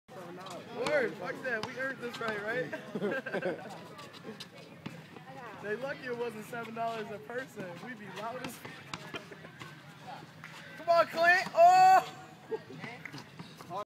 Basketball at green lakes
Jay, clint, Seth, Sam, mike, basketball at green lakes